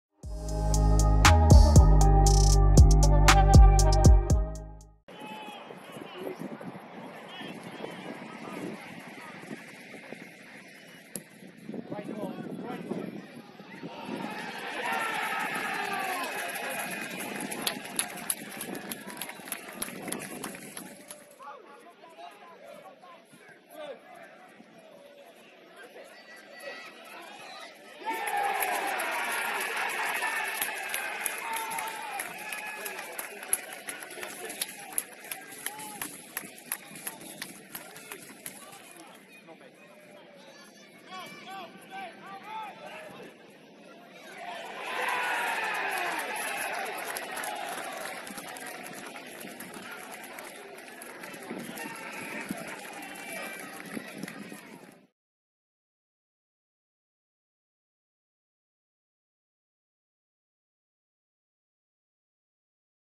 Goals | Weston-Super-Mare (A) - National League South
Goals from Chelmsford's 3-0 win against Weston-Super-Mare.

Footage filmed by Weston-Super-Mare AFC
Highlights edited by Josh Lowe